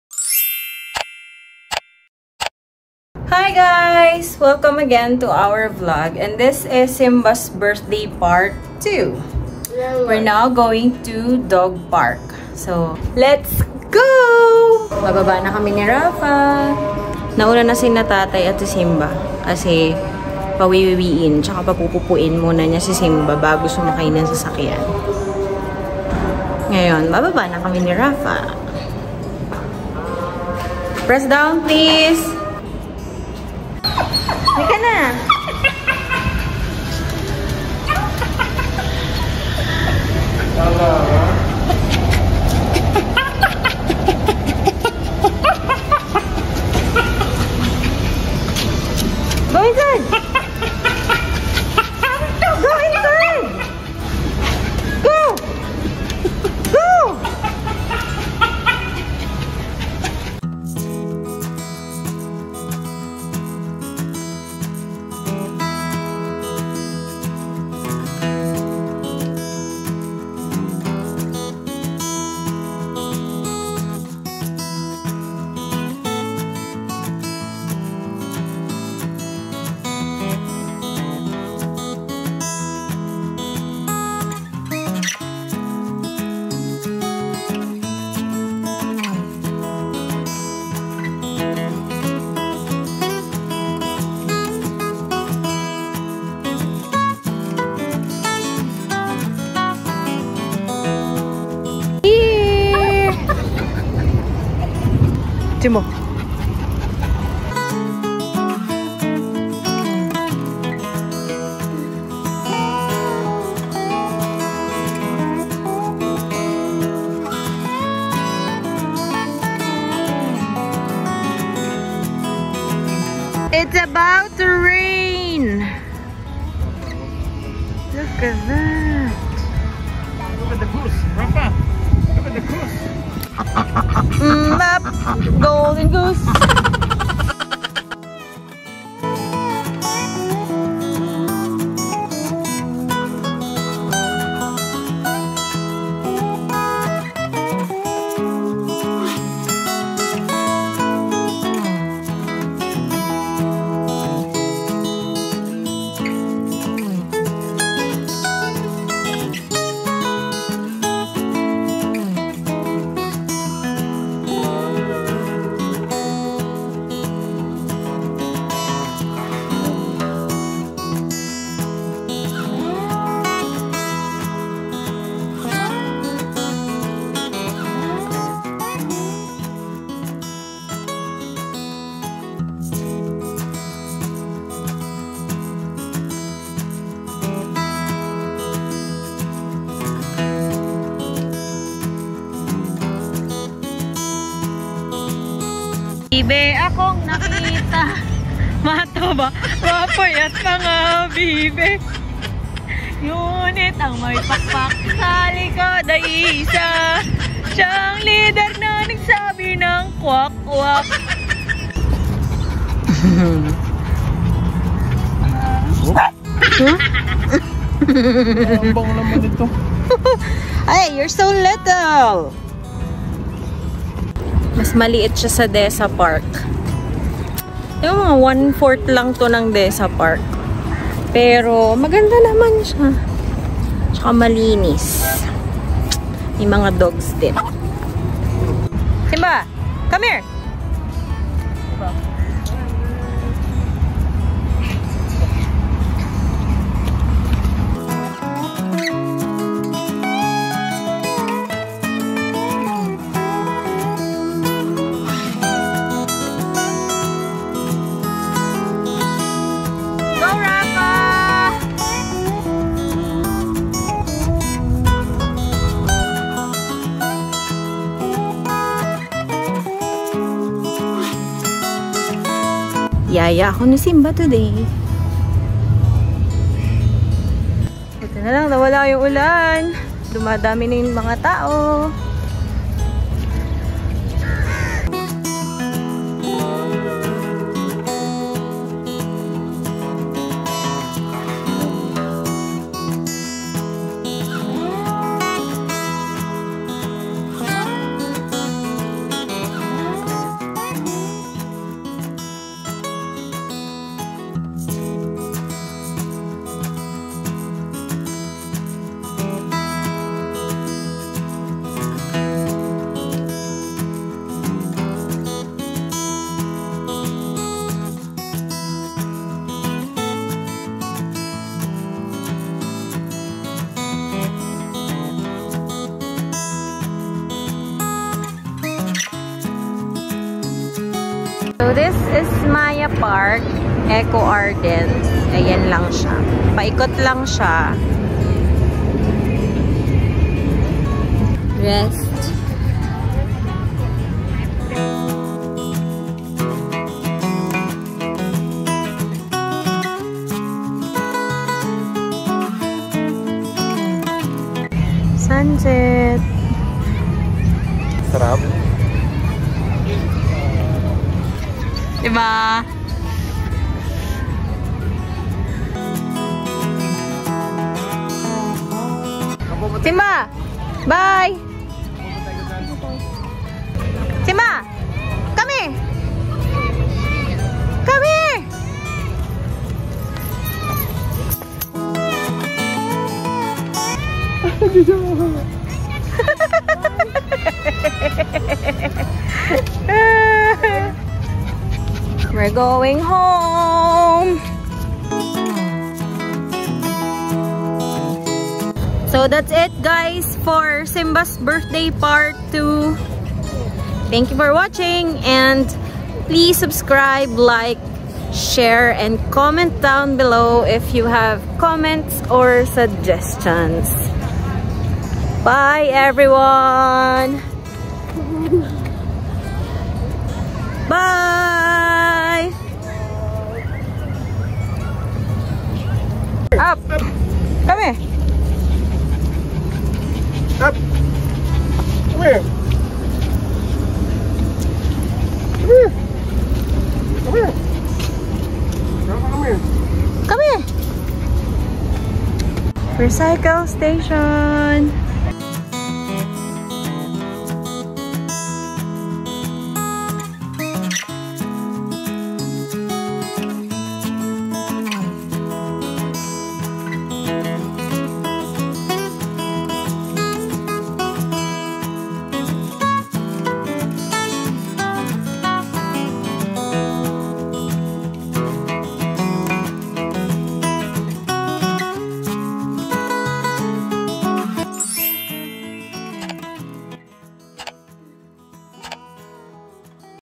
Maya Park Eco Ardence | Simba's Birthday Part 2 | Setia Alam Park | Pet Friendly | Shah Alam Vlog
Hi everyone! In this vlog, we celebrate our fur baby's 5th barkday. This is part 2 of his birthday celebration. We hope you enjoy watching this ❤️